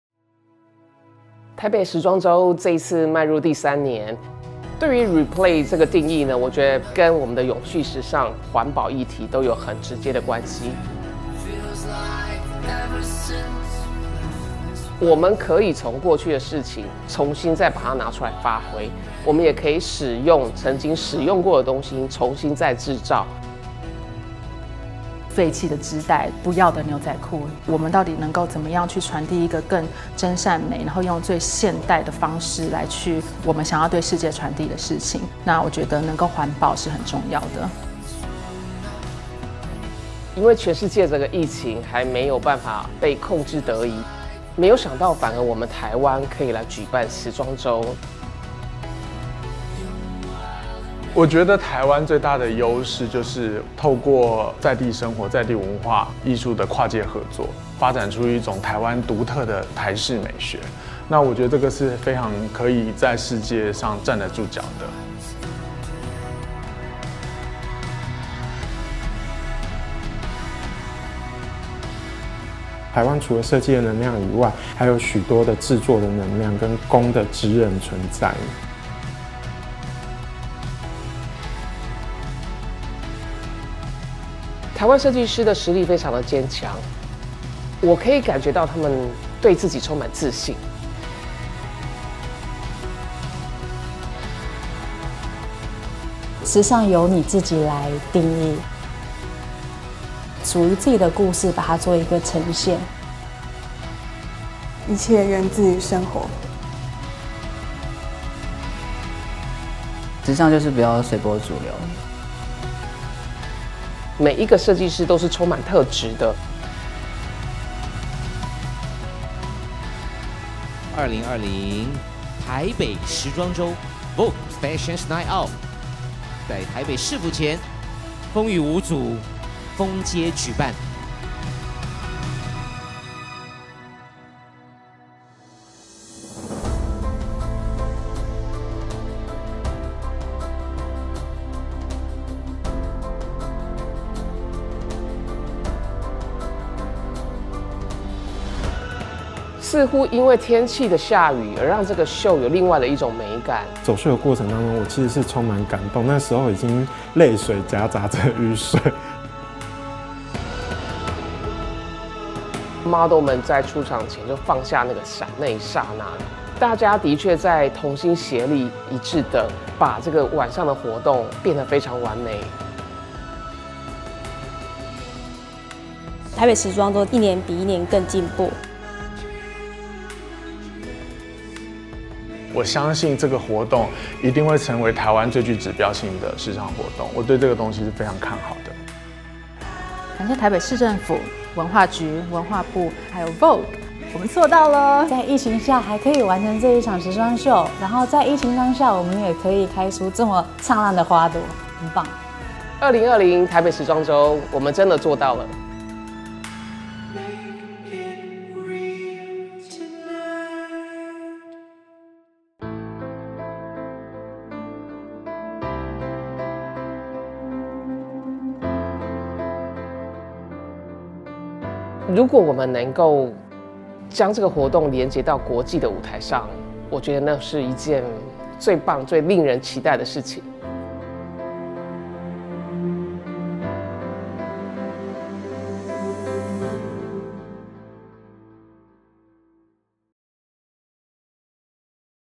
2020臺北時裝週 X VOGUE FNO全記錄
《2020臺北時裝週》壓軸時尚大秀 10/17臺北市府廣場磅礡登場！臺北市政府文化局、VOGUE、LEXUS聯手打造史無前例最高規格封街時尚大秀，新銳與國際級臺灣設計師聯手時裝展演，最強明星陣容齊聚朝聖臺北時尚不夜城、金曲歌王林宥嘉驚喜壓軸獻唱，雙時尚天后徐若瑄攜手林依晨氣勢現身、超人氣嘻哈男神瘦子E.SO與 DJ Afro時尚跨界開場，最受矚目潛力新星吳卓源與Kimberley驚艷演出。